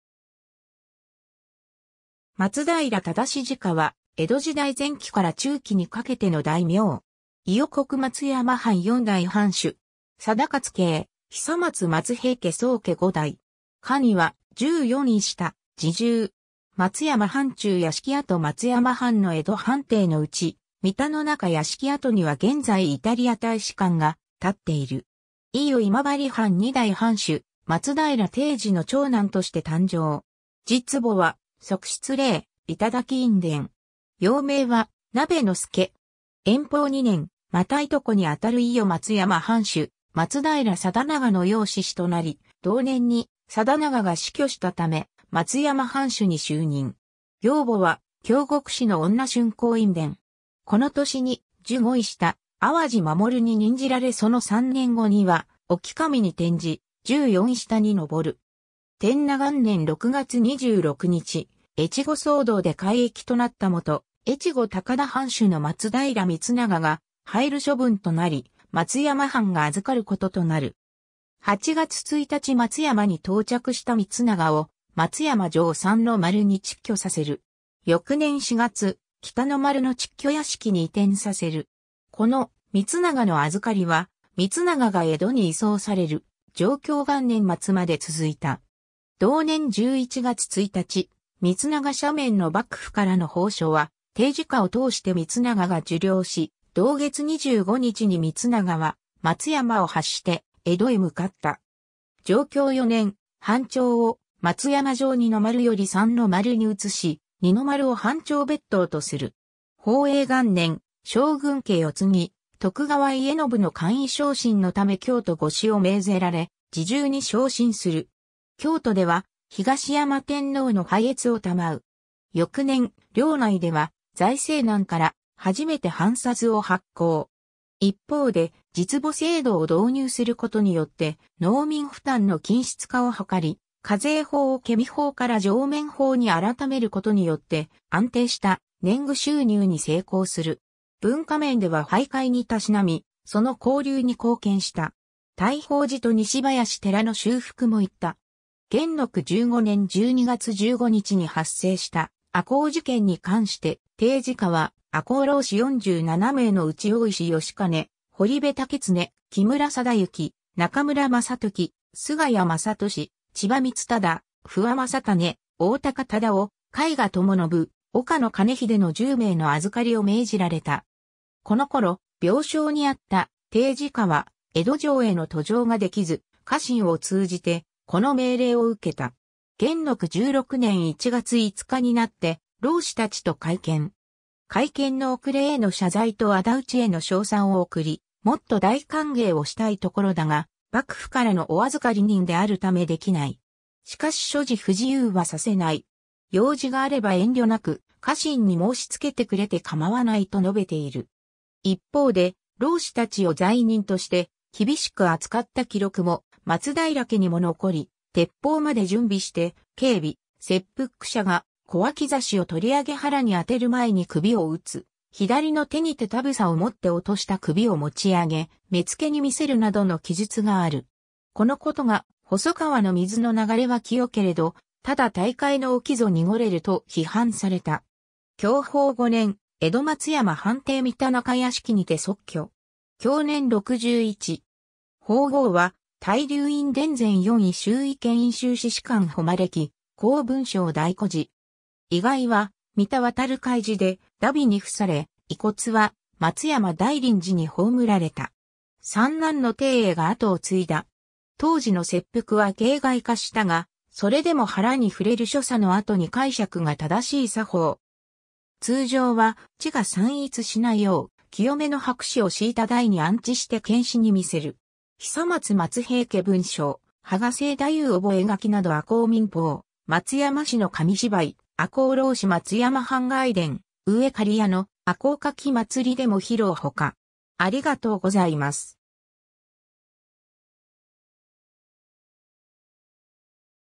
松平定直
松平 定直（まつだいら さだなお）は、江戸時代前期から中期にかけての大名。
伊予国松山藩4代藩主。
定勝系久松松平家宗家5代。
官位は従四位下・侍従。
 松山藩中屋敷跡（東京都港区） 松山藩の江戸藩邸のうち、三田の中屋敷跡には現在イタリア大使館が建っている。
 伊予今治藩2代藩主・松平定時の長男として誕生。
実母は側室嶺頂院殿（平岡氏の娘）。
幼名は鍋之助。
延宝2年（1674年）、又従兄に当たる伊予松山藩主・松平定長の養嗣子となり、同年に定長が死去したため松山藩主に就任。
養母は京極氏の女春光院殿。
この年に従五位下淡路守に任じられその3年後には隠岐守に転じ、従四位下に昇る。
 天和元年（1681年）6月26日、越後騒動で改易となった元・越後高田藩主の松平光長が配流処分となり、松山藩が預かることとなる。
8月1日松山に到着した光長を、松山城三ノ丸に蟄居させる。
翌年4月、北の丸の蟄居屋敷に移転させる。
この光長の預かりは、光長が江戸に移送される貞享元年(1683年)末まで続いた。
同年11月1日、光長赦免の幕府からの奉書は、定直を通して光長が受領し、同月25日に光長は松山を発して江戸へ向かった。
 貞享4年（1687年）、藩庁を松山城二ノ丸より三ノ丸に移し二ノ丸を藩庁別棟（隠居所）とする。
宝永元年（1704年）、将軍家世嗣徳川家宣の官位昇進のため京都御使を命ぜられ侍従に昇進する。
京都では東山天皇の拝謁を賜う。
翌年、領内では財政難から初めて藩札を発行。
一方で地坪制度を導入することによって農民負担の均質化をはかり、課税法を検見法から定免法に改めることによって安定した年貢収入に成功する。
文化面では俳諧にたしなみ、その興隆に貢献した。
大宝寺と西林寺の修復も行った。
 元禄15年12月15日（1703年1月31日）に発生した赤穂事件に関して、定直は赤穂浪士47名のうち大石良金・堀部武庸・木村貞行・中村正辰・菅谷政利・千馬光忠・不破正種・大高忠雄・貝賀友信・岡野包秀の10名の預かりを命じられた。
この頃、病床にあった定直は江戸城への登城ができず家臣を通じてこの命令を受けた。
元禄16年1月5日（1703年2月20日）になって浪士達と会見。
会見の遅れへの謝罪と仇討ちへの称賛を送り、「もっと大歓迎をしたいところだが、幕府からのお預かり人であるためできない。しかし諸事不自由はさせない。用事があれば遠慮なく家臣に申し付けてくれてかまわない」と述べている。
 一方で、浪士たちを罪人として厳しく扱った記録も松平家にも残り、「鉄砲まで準備して警備」「切腹者が小脇差を取り上げ腹に当てる前に首を打つ」「左の手にて髻（たぶさ）を持って落とした首をもち上げ、目付に見せる」などの記述がある。
 このことが「細川の　水の（水野忠之）流れは清けれど　ただ大海（毛利甲斐守）の　沖（松平隠岐守）ぞ濁れる」（当時の狂歌）と批判された。
（それでも、近年は松山市を義士会が訪れ赤穂義士祭が行われるなど赤穂市と交流がある。） 享保5年（1720年）、江戸松山藩邸三田中屋敷にて卒去。
 法号は大龍院殿前四位拾遺兼隠州刺史観誉喜広聞証大居士。
遺骸は三田済海寺で荼毘に付され、遺骨は松山大林寺に葬られた。
三男の定英が跡を継いだ。
 ^ 当時の切腹は形骸化したが、それでも腹に触れる所作（実際には切らない）の後に介錯が正しい作法。
 ^ 通常は血が散逸しないよう、清めの拍紙を敷いた台に安置して検使に見せる。
 ^ 久松松平家文書「波賀清太夫覚書」など ^ 赤穂民報「松山市の紙芝居「赤穂浪士松山藩外伝」、上仮屋の「赤穂かきまつり」でも披露」ほか